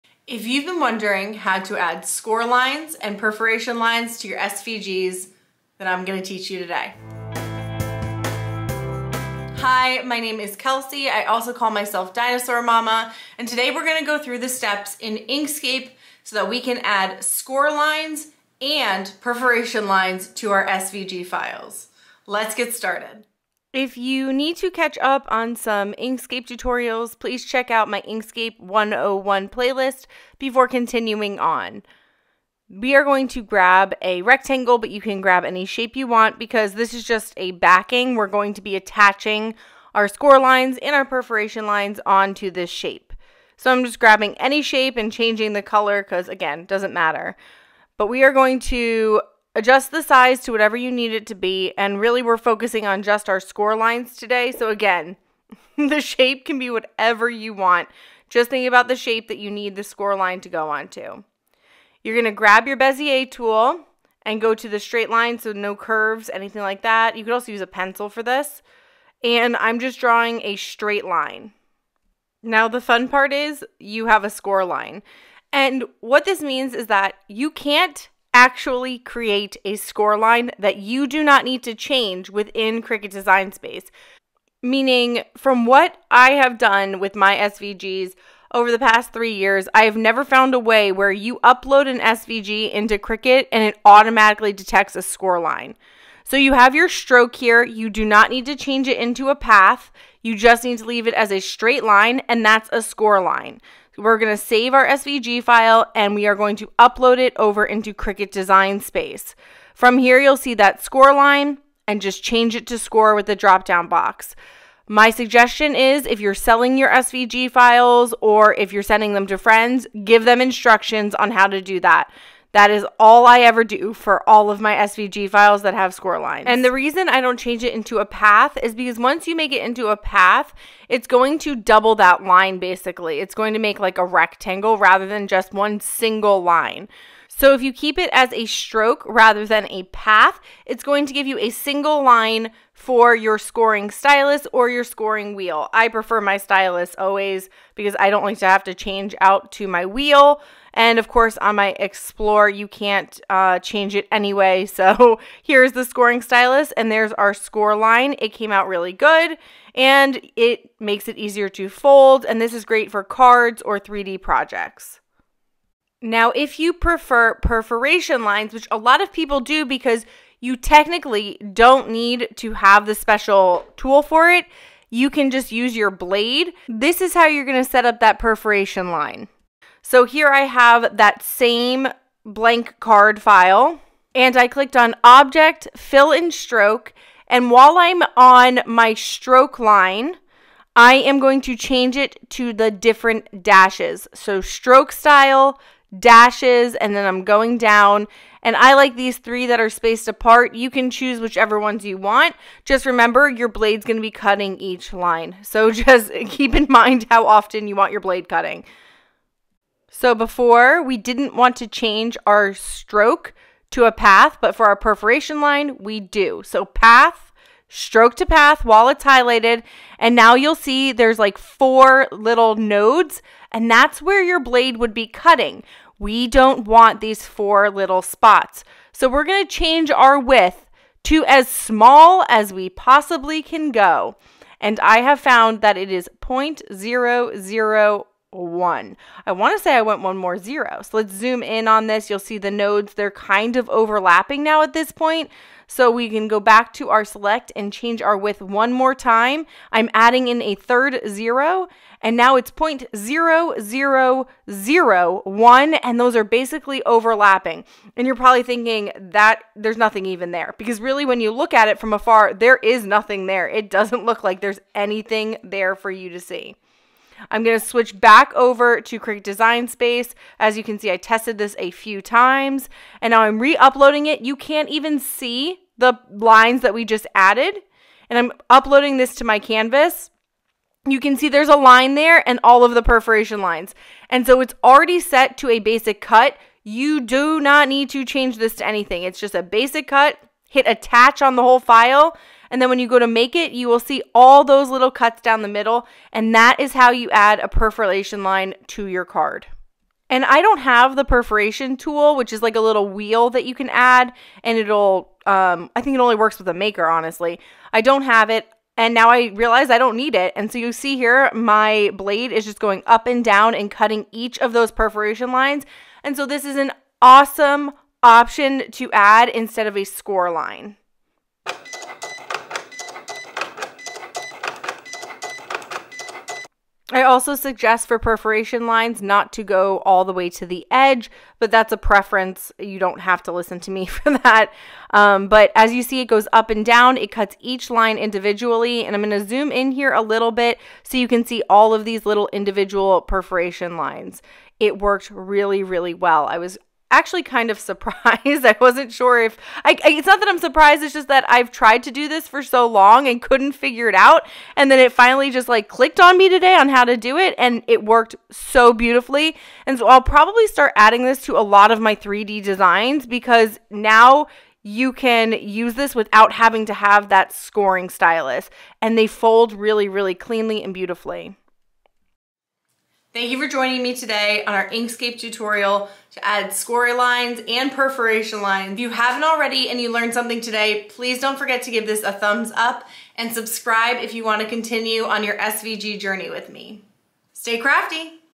Add Score Lines & Perforation Lines in Inkscape for EASY 3D & Card Designs with Cricut! 🫶 DIY SVGs
If you've been wanting to make your own SVG files and you're wondering how to add score lines and perforation lines to those SVG files for free in Inkscape then this tutorial will walk you through step-by-step to make Cricut files for free in Inkscape.

If you're just starting your SVG journey make sure to check out my escape 101 playlist on YouTube as well as my blog and website for free SVG files